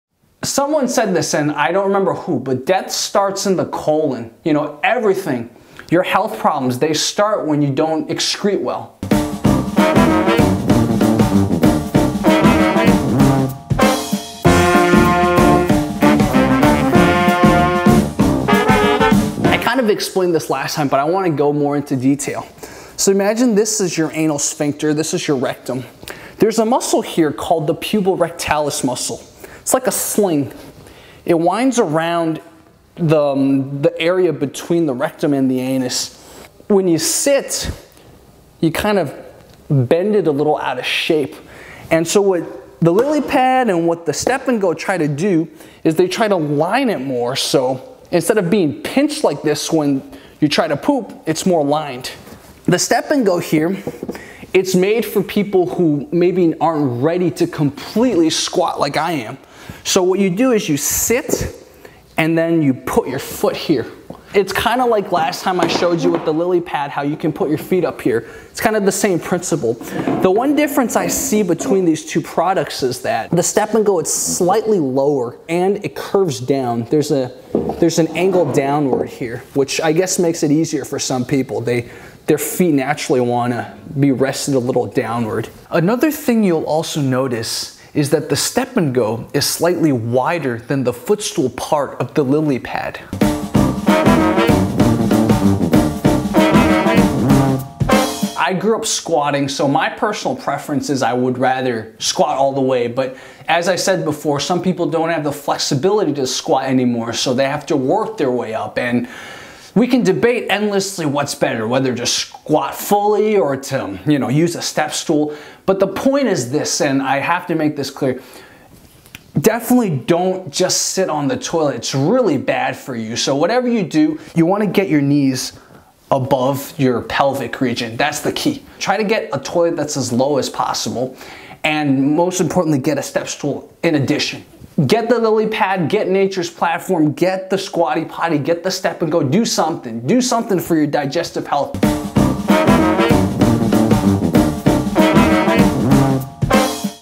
Step And Go Step Stool Review - Health Product Comparison (vs Lillipad)
This is a great product that Bud sent me in 2015. Our Western way of sitting on the toilet is extremely bad for our digestive system, so this product is one of the ways entrepreneurs have tried to get us around our non-ergonomic toilets. I want to thank Koba and Zurab for helping me make this review.

What should I try or review next?

Timecodes:
0:00 Intro
0:19 Anatomy lesson
0:52 Step and go's principle
1:10 Step and go vs Lillipad
1:26 More differences
1:38 Final thoughts